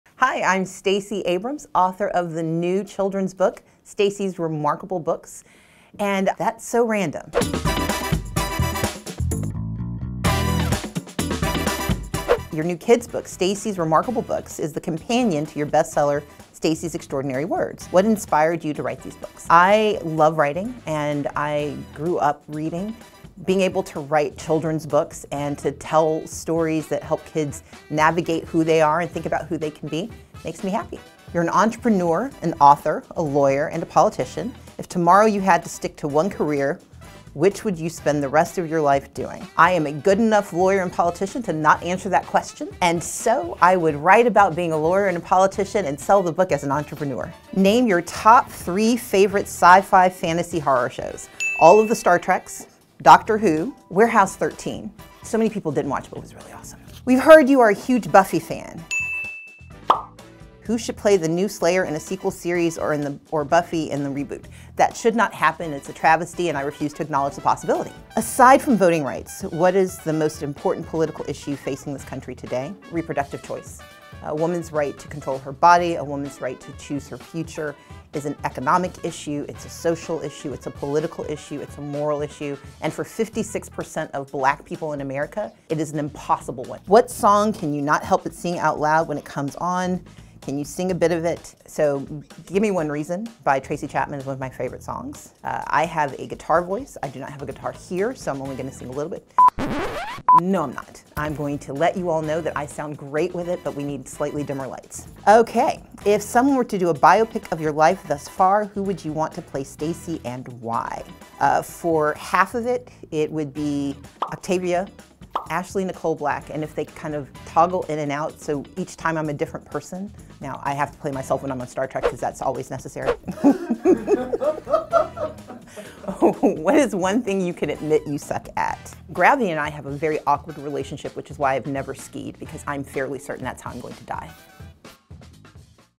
Voting Rights Advocate, Stacey Abrams, Plays That's So Random
Stacey Abrams plays That's So Random. The former Georgia gubernatorial candidate talks about her new kids book, "Stacey's Remarkable Books," geeks out on sci-fi/fantasy, and more.